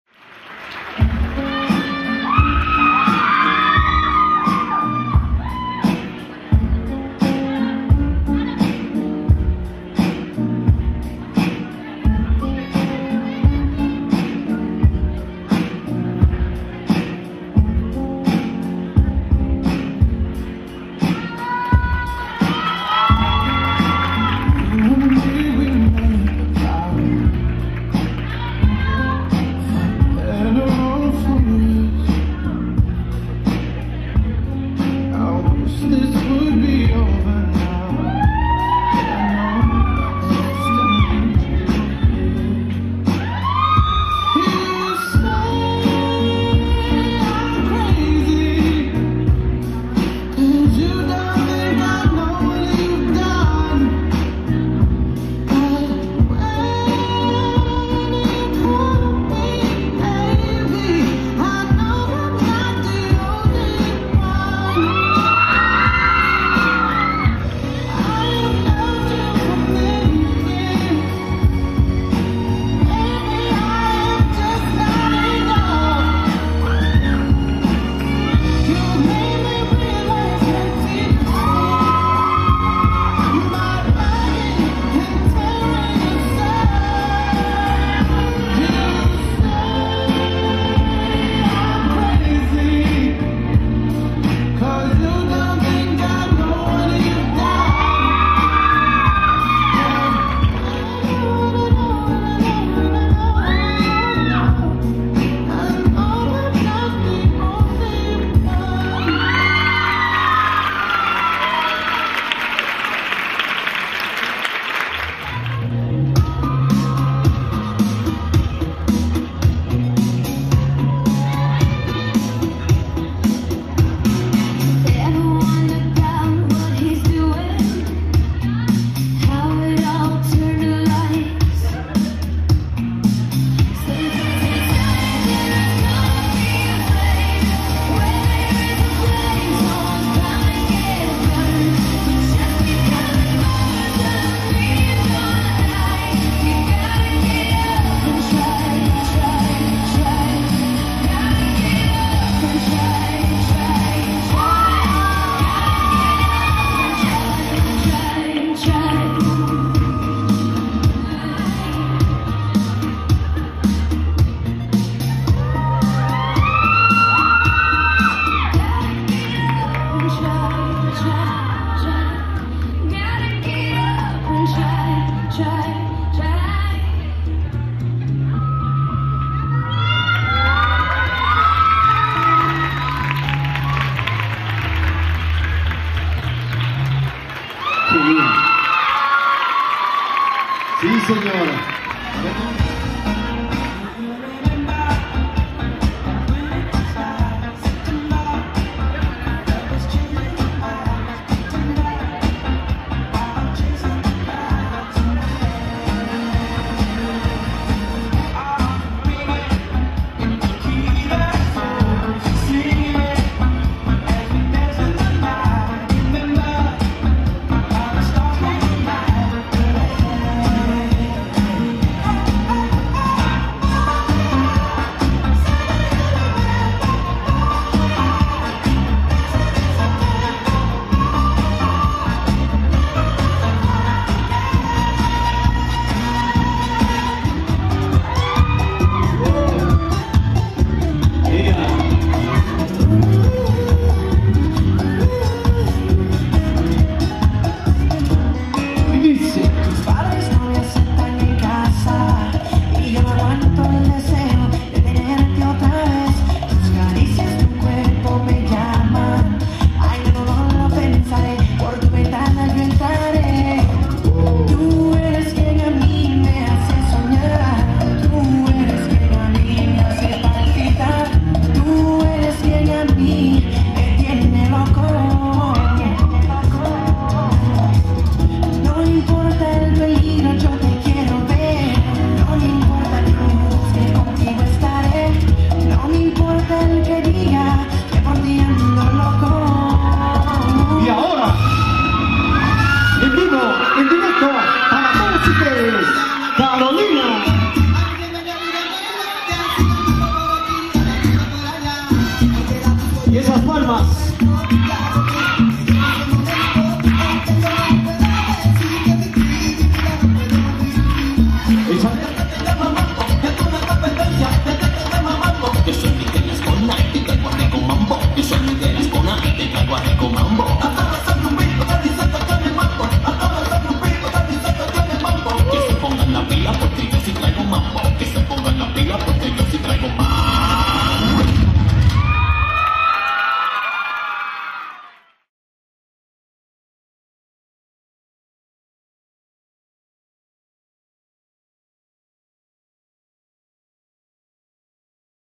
15 Años Vals Interactivo Karolina Salones Alvi Politécnico Mirador Audio, Foto y Video Zon Caribe
Evento efectuado en los Salones Alvi Politécnico Salón Mirador en la Delegación Gustavo A. Madero en la Ciudad de México.

Vals Interactivo con pantallas de proyección y proyector.
Videos hechos en studio croma.
Academias de Baile Profesional.

Servicio Profesional de Fotografía y Video Filmaciones para Bodas, 15 Años, Empresariales, Renta de Pantallas, Circuito Cerrado CCTV, etc.